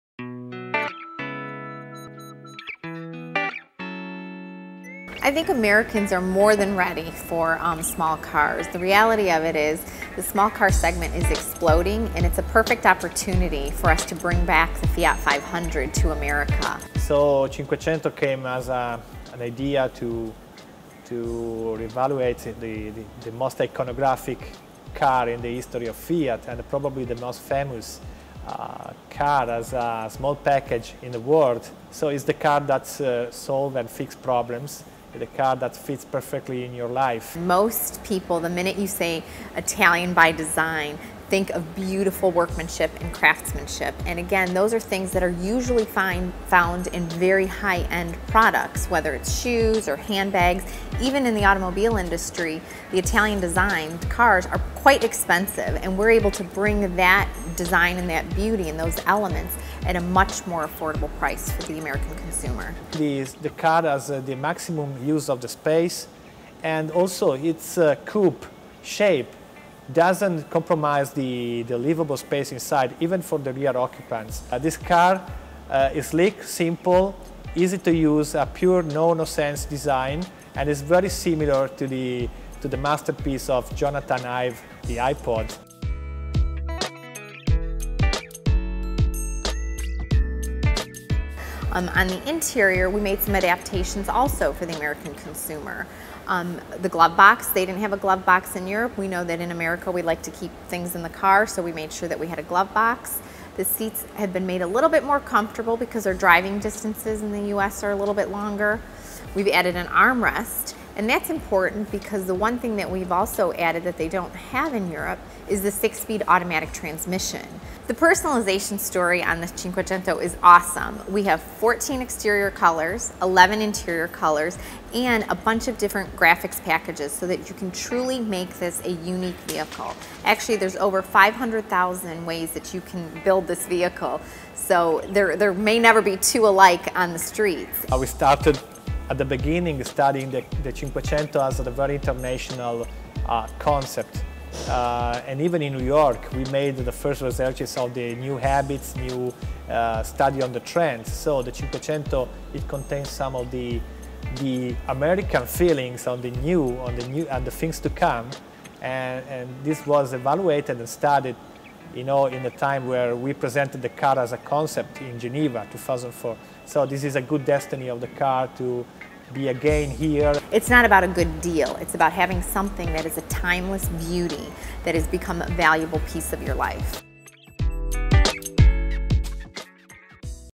Fiat Evolution
- Fiat Brand North America - An introduction to the North American version of the 2012 Fiat 500, with running footage, photos and comments from Laura Soave, Head, Fiat North America, and Roberto Giolito, Head, Fiat Style.